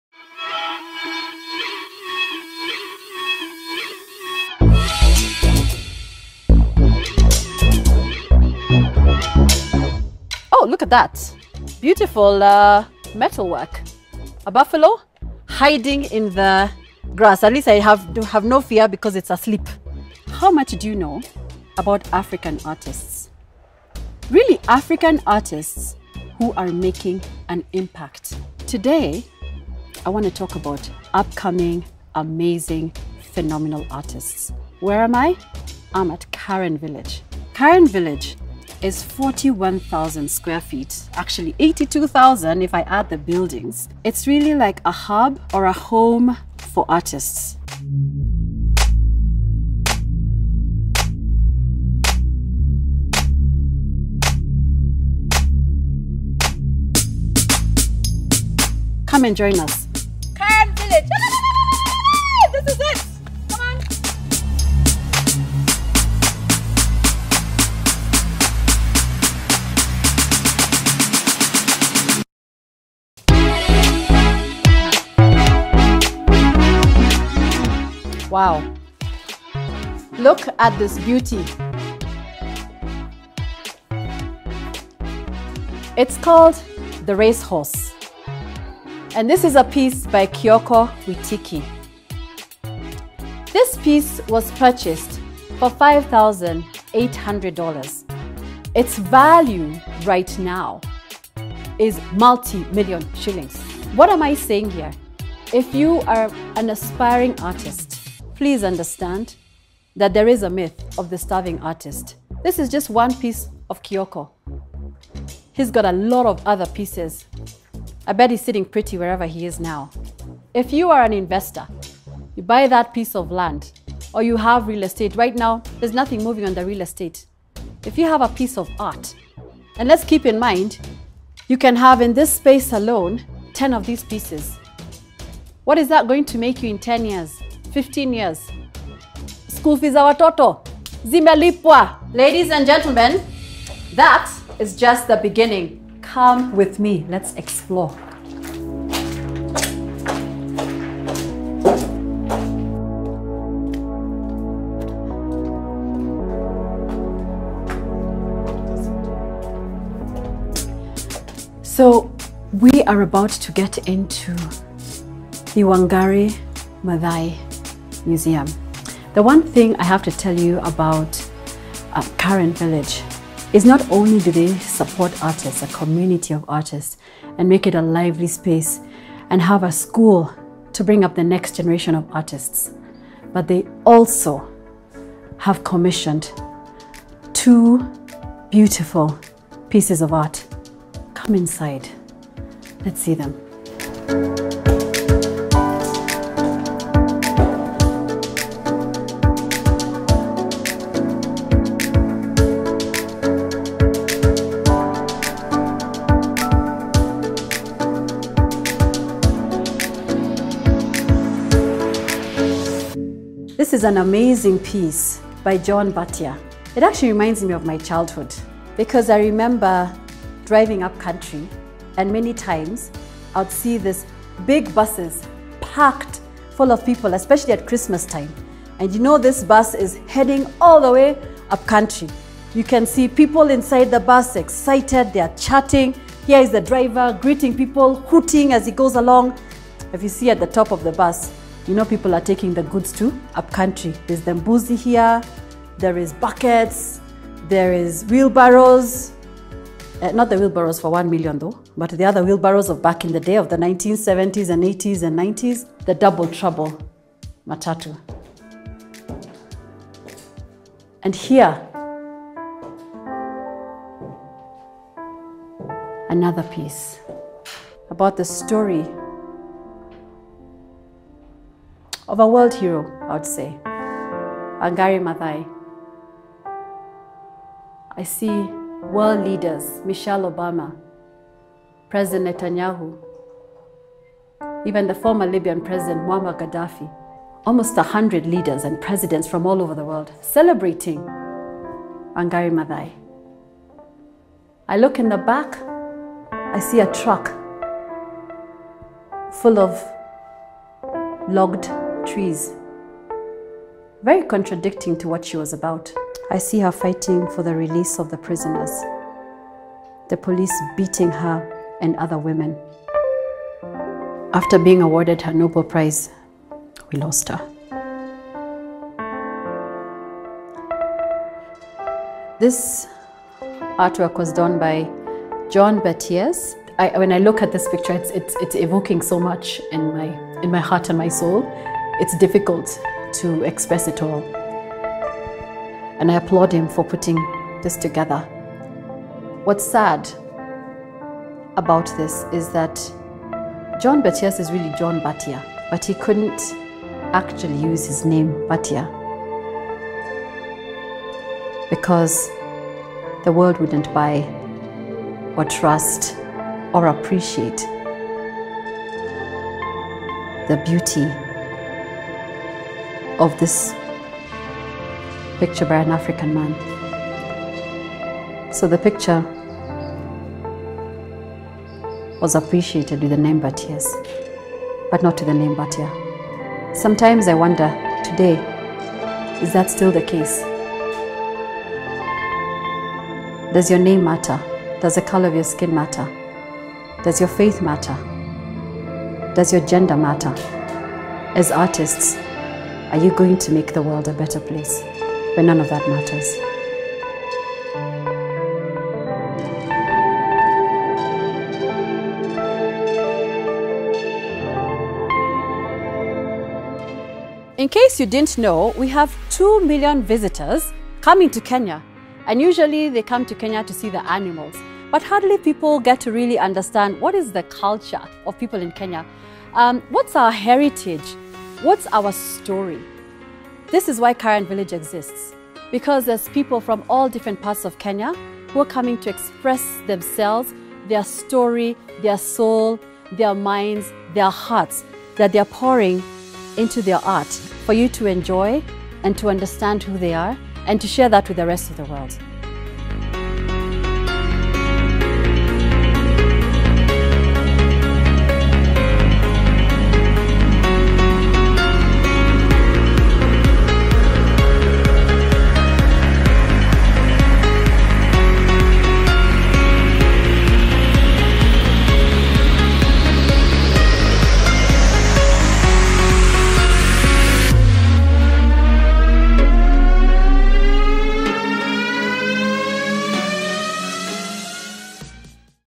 Karen Village Tour - See the art!
Kabuki Anyumba gives us a tour at Karen Village introducing us to artists and commissioned art at the Village. @Zan_Artts @artcultureheritageafrica @karenvillage @KarenVillageNBI